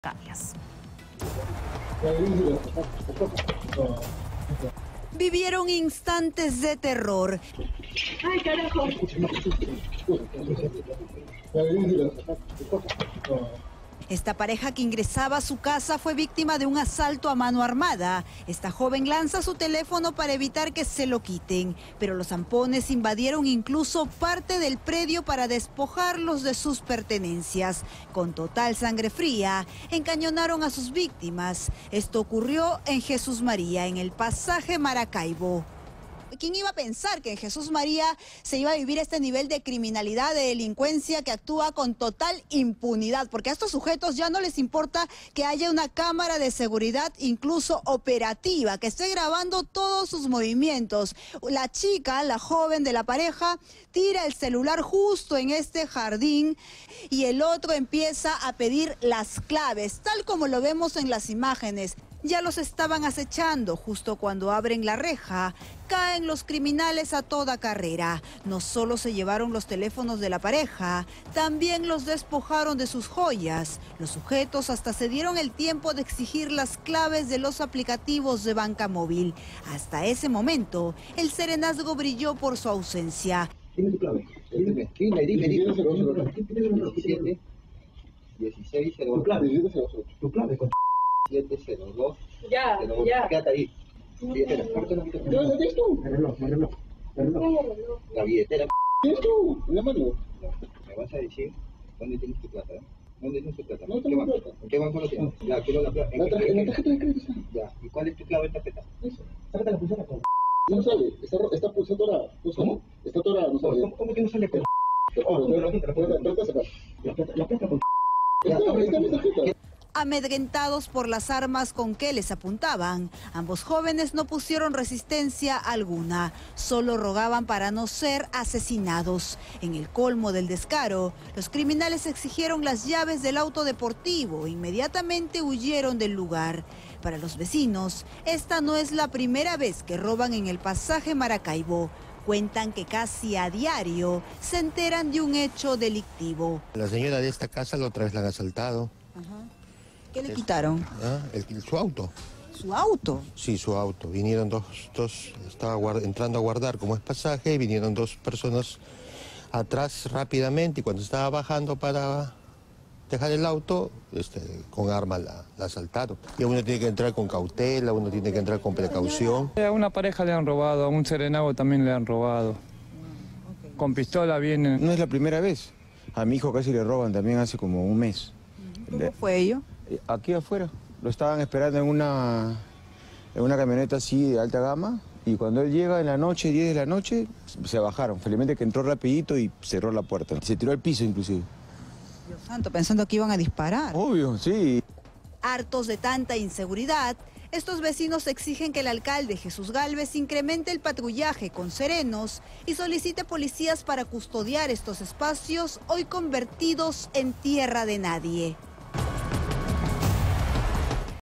Jesús María: vecinos piden mayor vigilancia tras robo a pareja en puerta de su casa
La criminalidad sigue ganando terreno en Lima. Esta vez, dos delincuentes abordaron a una pareja en la puerta de su casa en Jesús María y terminaron despojándolos de todas sus pertenencias.

En las imágenes de las cámaras de seguridad, se observa a uno de los hampones con un arma de fuego apuntando a sus víctimas, mientras que su cómplice comienza a revisar los bolsillos de los enamorados.

Al verse rodeada por los hampones, la mujer que estaba con el celular en la mano decidió arrojar su teléfono al jardín. El móvil de su pareja fue encontrado por uno de los delincuentes, quién le pidió la clave de su cuenta bancaria, con la finalidad de quitarle todo su dinero.

MAYOR VIGILANCIA

Tras este hecho, los vecinos de Jesús María exigieron a las autoridades mayor despliegue de agentes de la PNP y serenazgo, debido a los constantes robos que se vienen registrando en la jurisdicción.